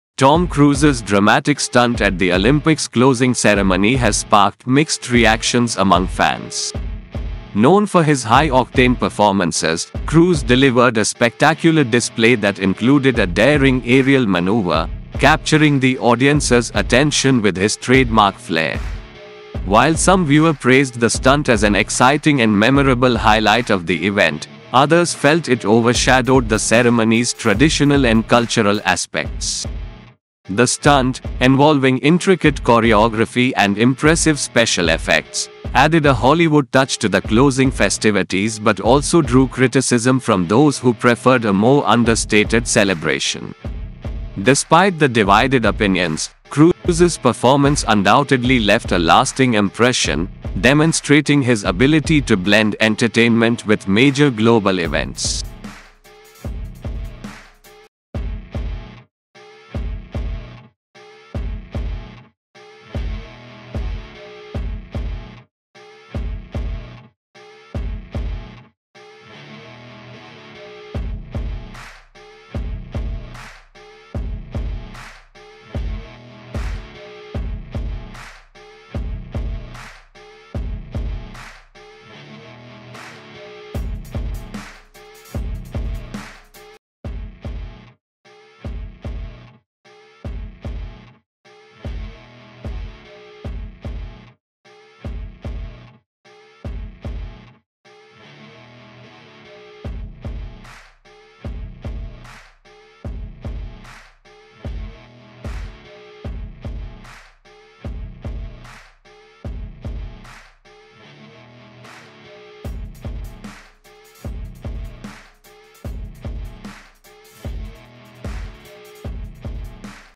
Tom Cruise divides Olympics fans with dramatic stunt at closing ceremony
Tom Cruise's dramatic stunt at the Olympics closing ceremony has sparked mixed reactions among fans. Known for his high-octane performances, Cruise delivered a spectacular display that included a daring aerial maneuver, capturing the audience's attention with his trademark flair. While some viewers praised the stunt as an exciting and memorable highlight of the event, others felt it overshadowed the ceremony's traditional and cultural aspects. The stunt, involving intricate choreography and impressive special effects, added a Hollywood touch to the closing festivities but also drew criticism from those who preferred a more understated celebration. Despite the divided opinions, Cruise's performance undoubtedly left a lasting impression, demonstrating his ability to blend entertainment with major global events.